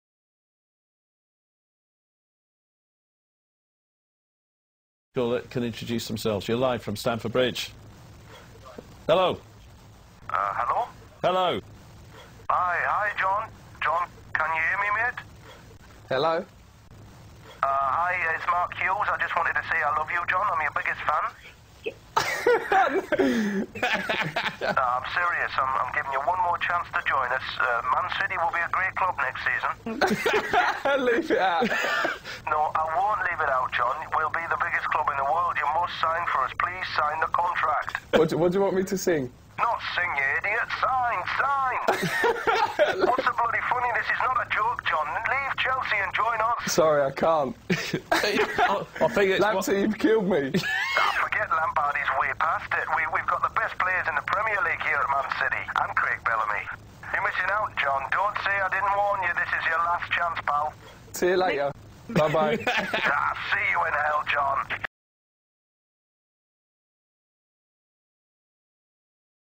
Mark Hughes attempts to presuade John Terry to go to Manchester City
NOTE: I'm a huge fan of City, I just find this funny so I had to upload it.